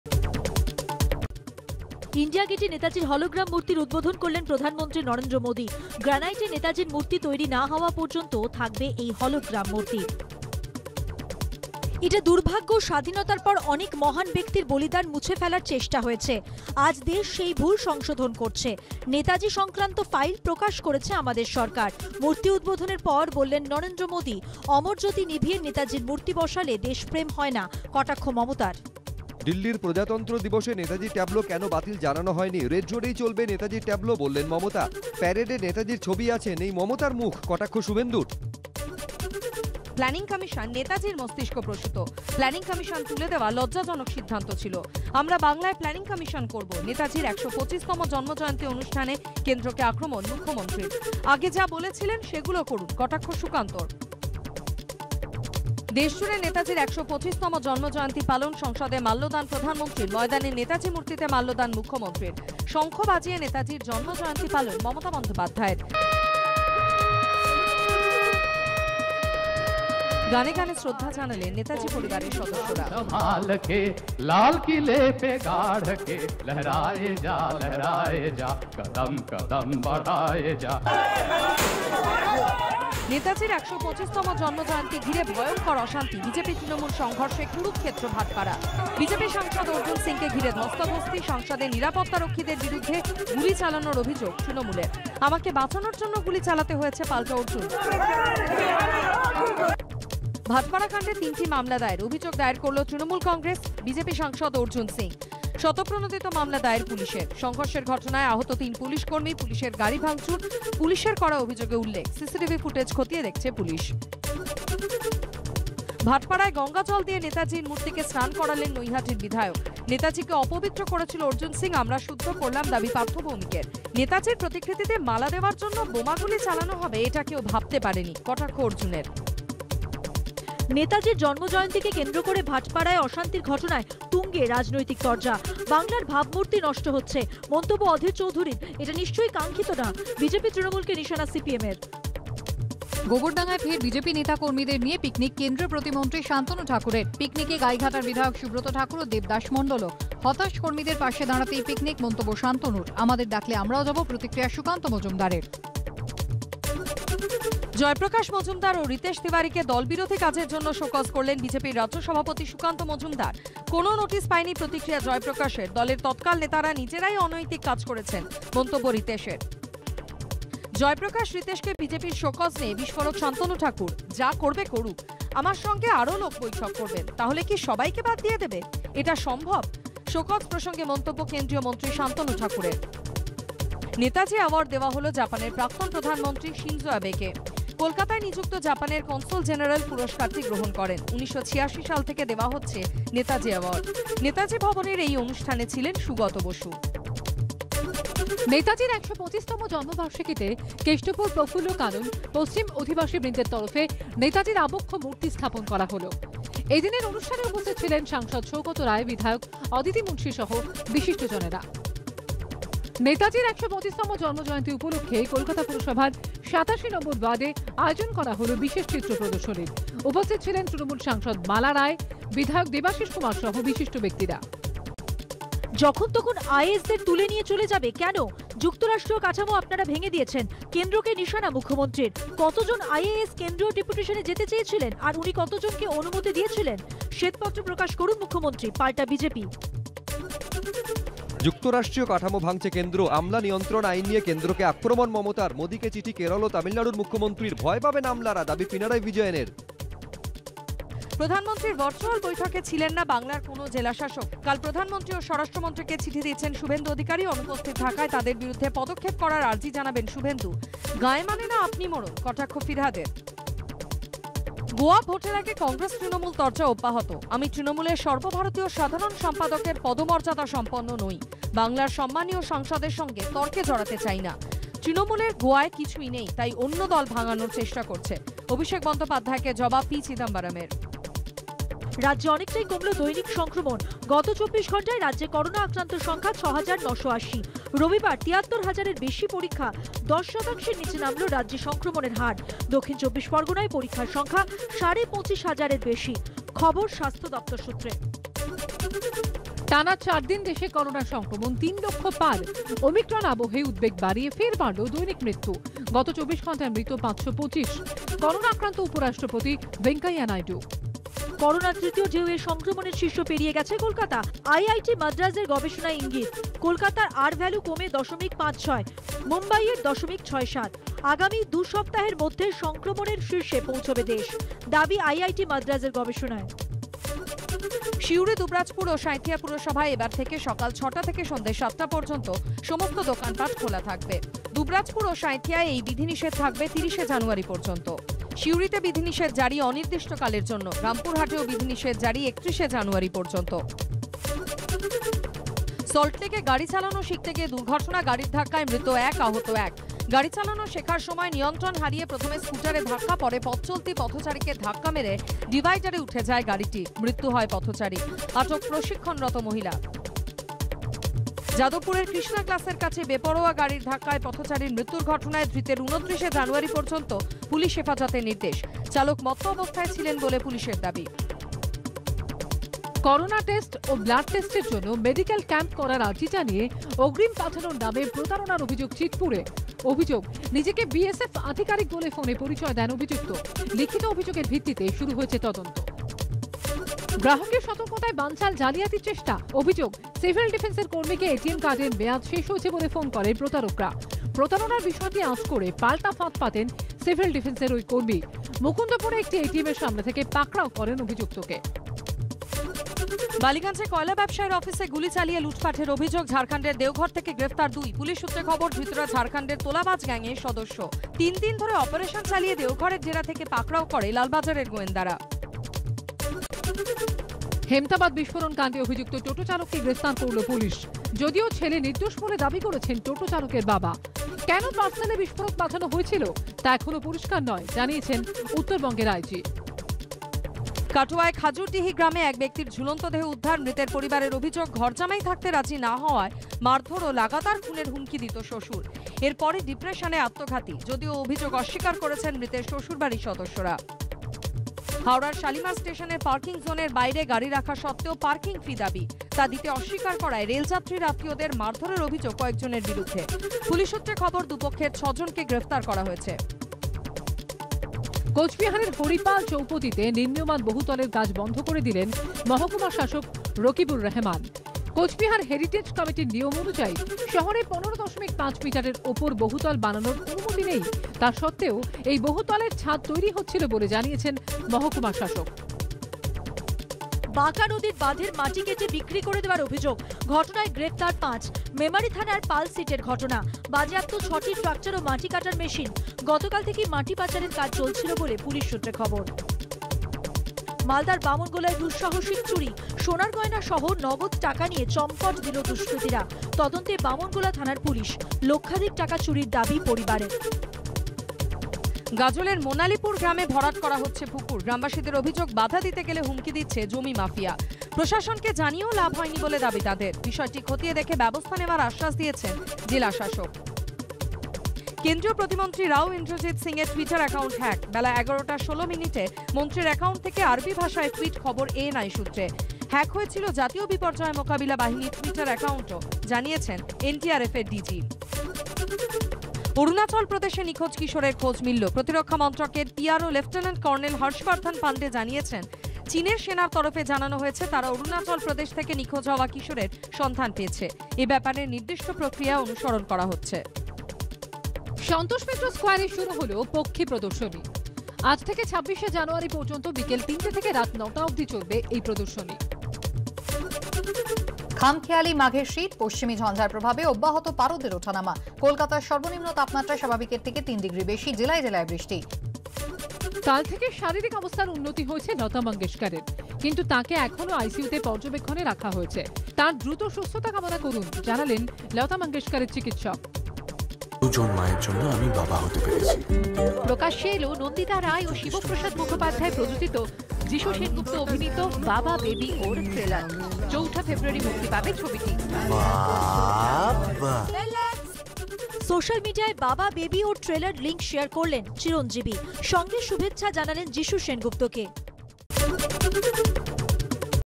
Speed News: নেতাজি সংক্রান্ত ফাইল প্রকাশ করেছে আমাদের সরকার, নেতাজির হলোগ্রাম মূর্তির উদ্বোধনে মোদি
ইন্ডিয়া গেটে নেতাজির হলোগ্রাম মূর্তির উদ্বোধন করলেন প্রধানমন্ত্রী নরেন্দ্র মোদি। গ্রানাইটের নেতাজির মূর্তি তৈরি না হওয়া পর্যন্ত থাকবে এই হলগ্রাম মূর্তি।

 এটা দুর্ভাগ্য স্বাধীনতার পর অনেক মহান ব্যক্তির বলিদান মুছে ফেলার চেষ্টা হয়েছে। আজ দেশ সেই ভুল সংশোধন করছে।  নেতাজি সংক্রান্ত ফাইল প্রকাশ করেছে আমাদের সরকার। মূর্তি উদ্বোধনের পর বললেন নরেন্দ্র মোদি। অমরজ্যোতি নিভিয়ে নেতাজির মূর্তি বসালেই দেশপ্রেম হয় না। কটাক্ষ মমতার।

দিল্লির প্রজাতন্ত্র দিবসে পশ্চিমবঙ্গের ট্যাবলো কেন বাতিল তা জানানো হয়নি। রেড রোডেই চলবে ট্যাবলো। বললেন মমতা।